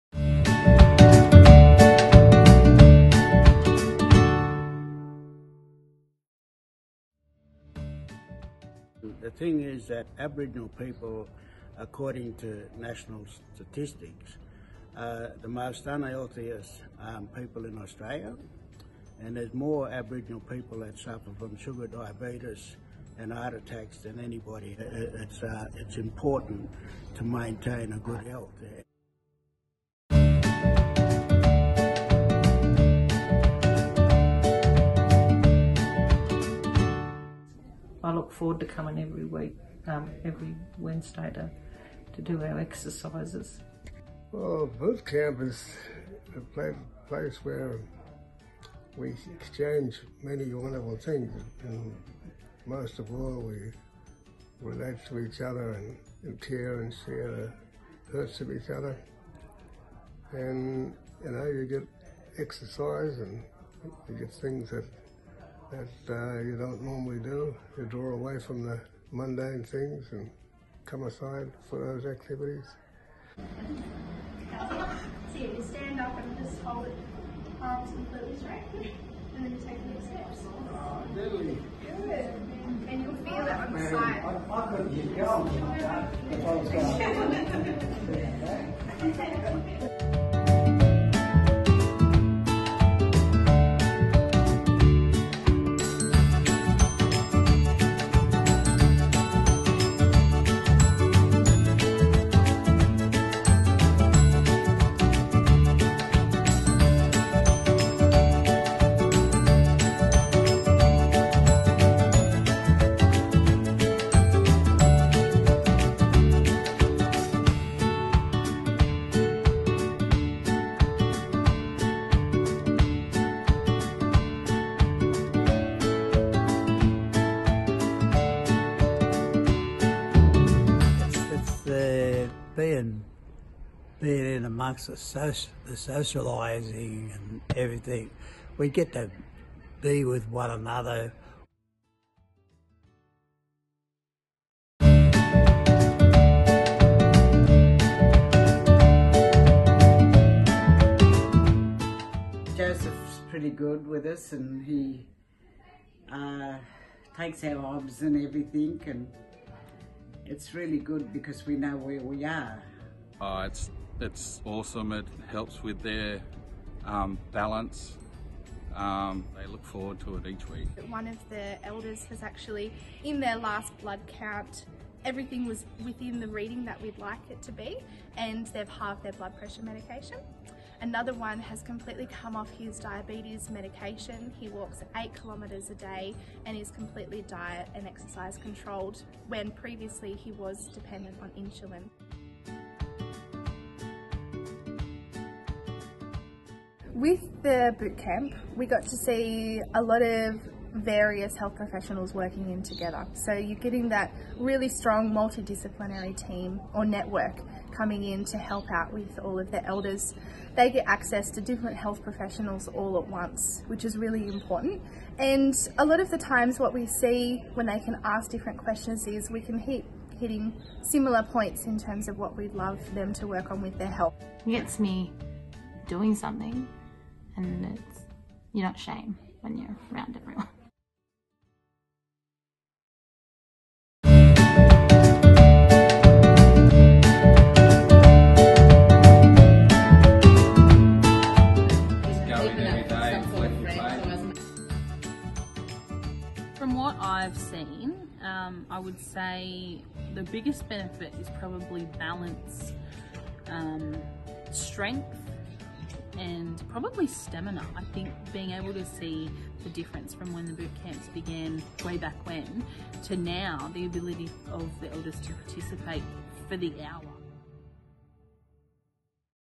koori Bootcamp
The GPH Koori Bootcamp in Eden was a great success story